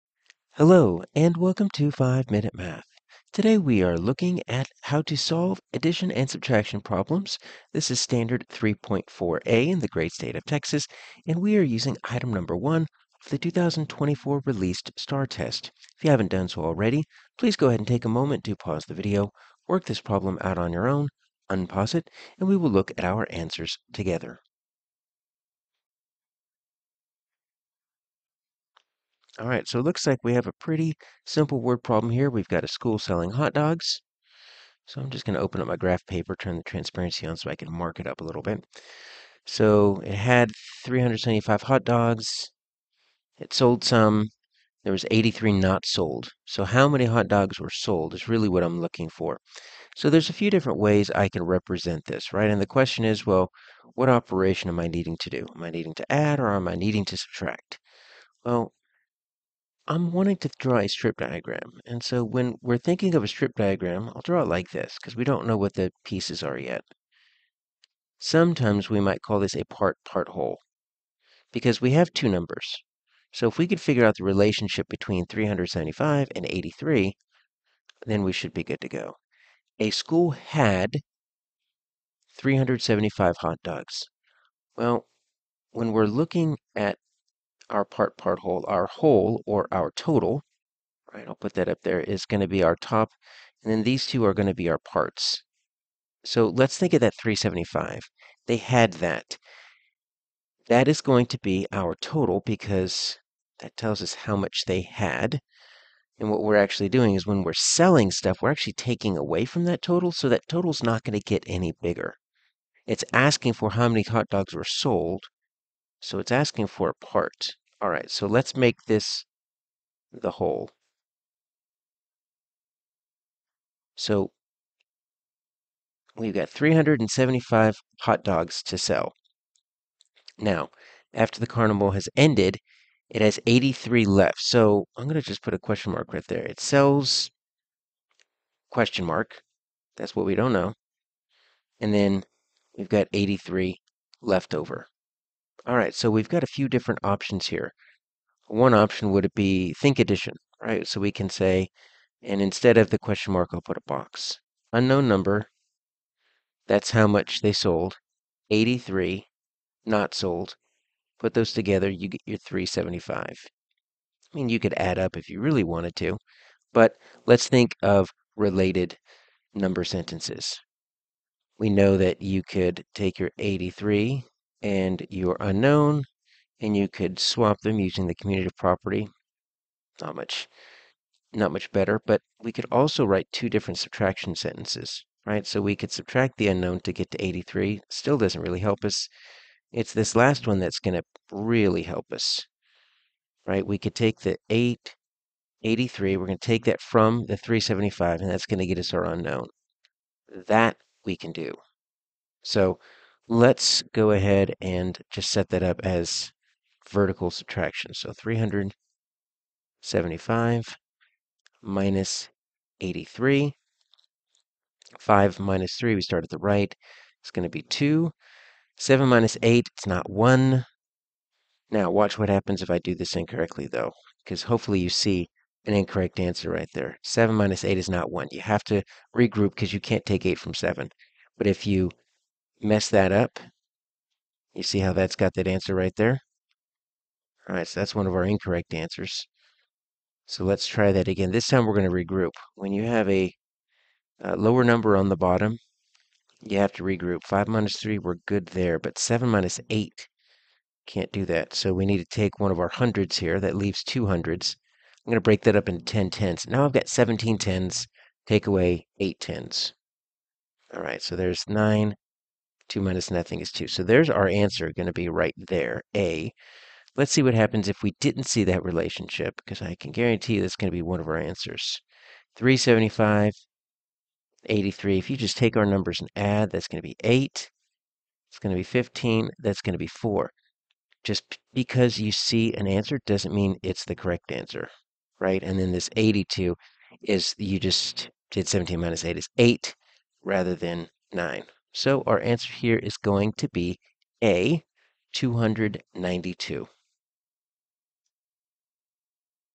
3rd Grade STAAR Practice Solving Addition and Subtraction Problems (3.4A - #18)
Practice solving with fluency one-step and two-step problems involving addition and subtraction within 1,000 using strategies based on place value, properties of operations, and the relationship between addition and subtraction with a test item from the 2024 STAAR test (#1 - 3.4A).